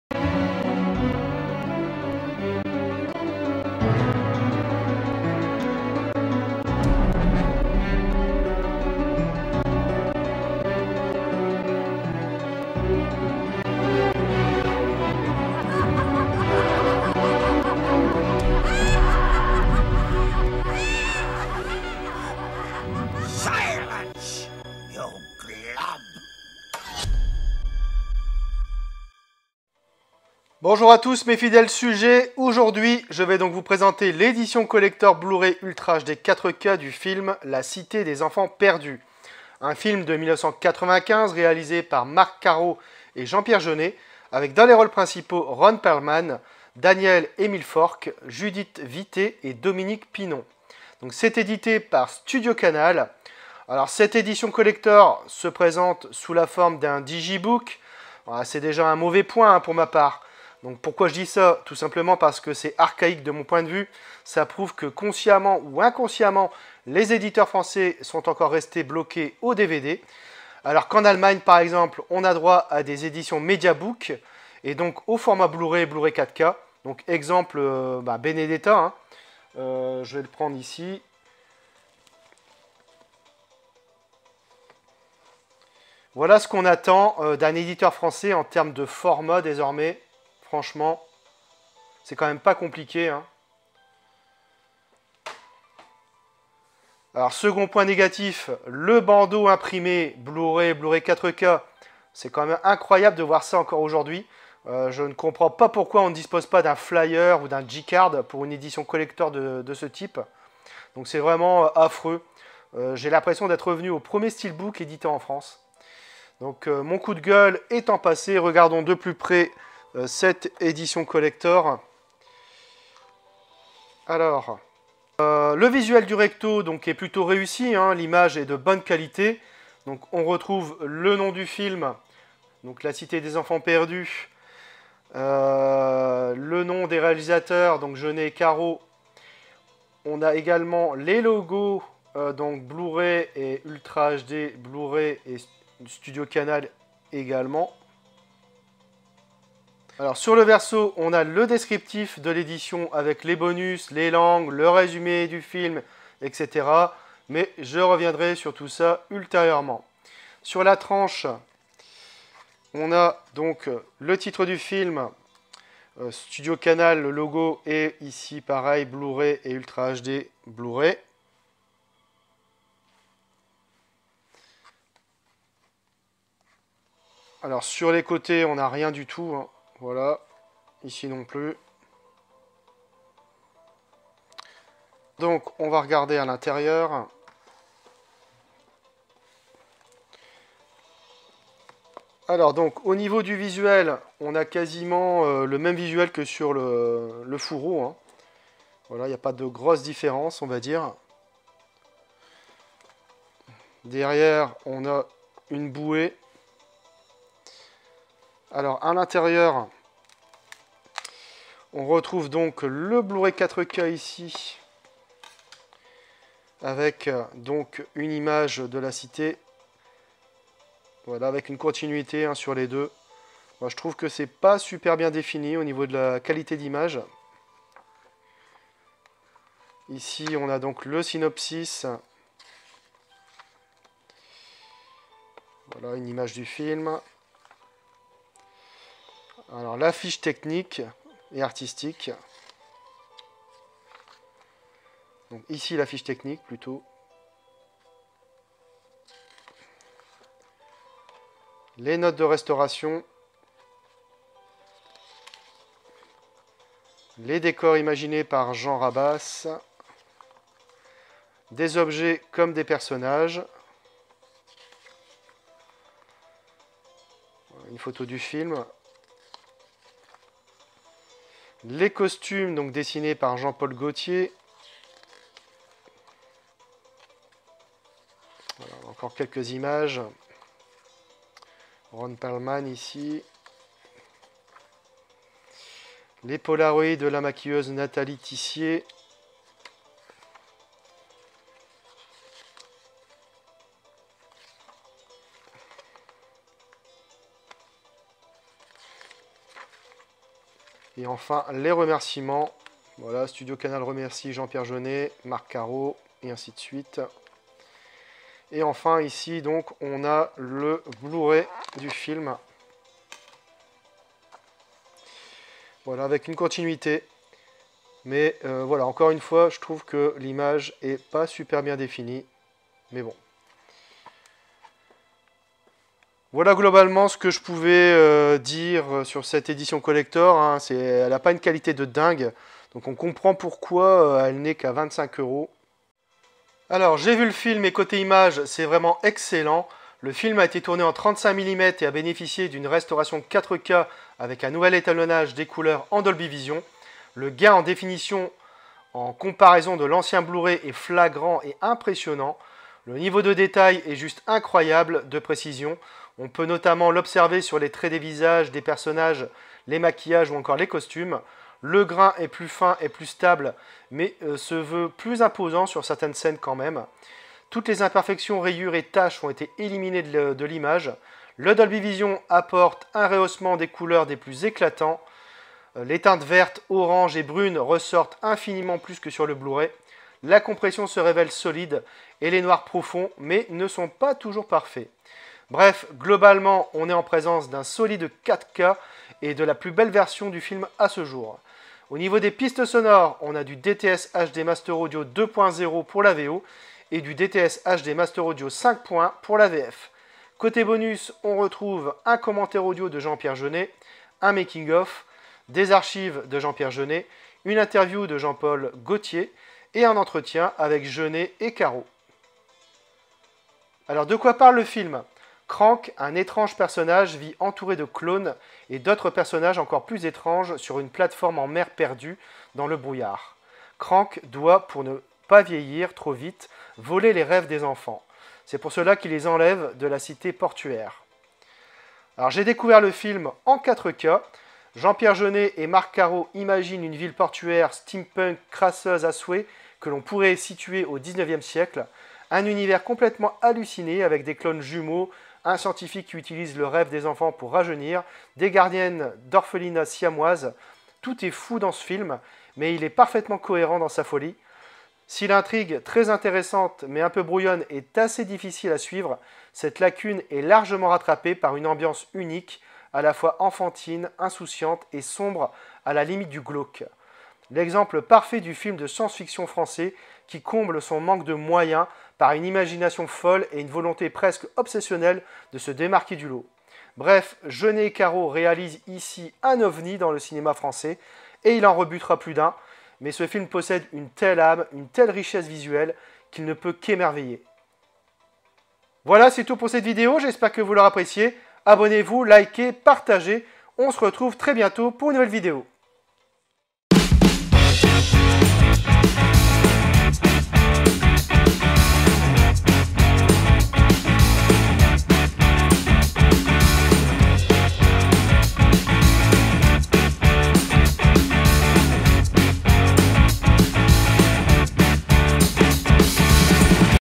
12 - LA CITÉ DES ENFANTS PERDUES ÉDITION COLLECTOR BLU-RAY 4K ULTRA HD UNBOXING
Votre Baron "préféré" vous présente l'édition collector Ultra HD 4K du film La Cité Des Enfants Perdues avec dans les rôles principaux : Ron Perlman, Daniel Emilfork, Judith Vittet et Dominique Pinon.

Liker, partager et laissez moi des commentaires.

J'y répondrais mes chers fidèles sujets ! :-)